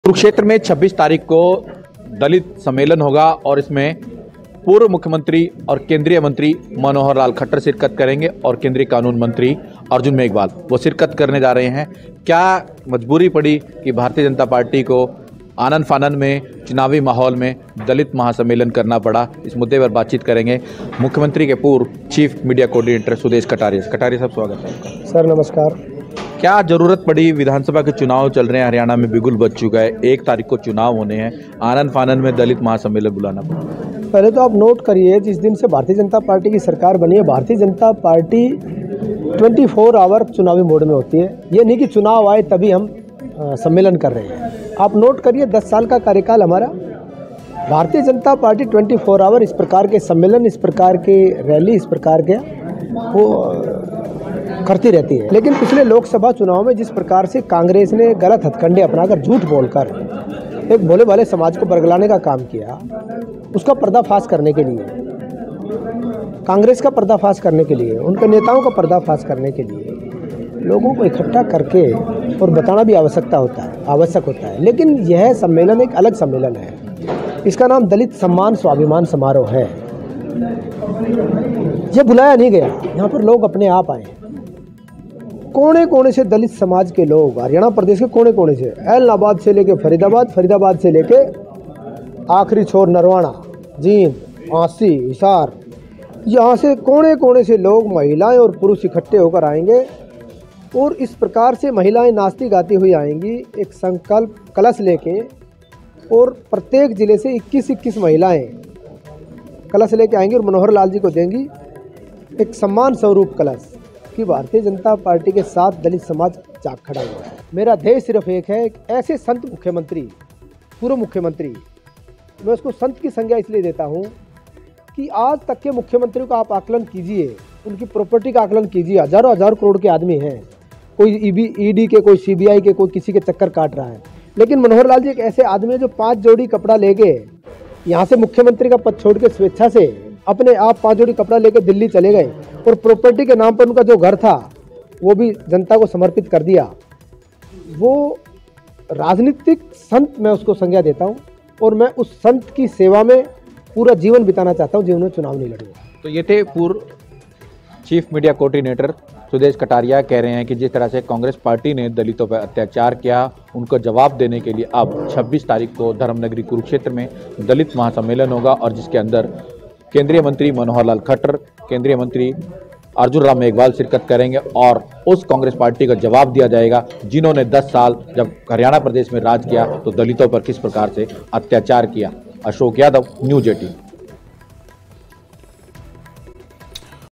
Haryana Election News | Kurukshetra में दलित सम्मेलन कल, पूर्व Chief Media Coordinator ने दी जानकारी
Dalit conference in Kurukshetra on 26th August.
Khattar will hold the 'lota of resolution' in the Mahasammelan of Dalits.
Former Chief Media Coordinator Sudesh Kataria gave information.

26 अगस्त को कुरुक्षेत्र में दलित सम्मेलन.
दलितों के महासम्मेलन में 'संकल्प का लोटा' पकड़ेगे खट्टर.
पूर्व चीफ मीडिया कोऑर्डिनेटर सुदेश कटारिया ने दी जानकारी.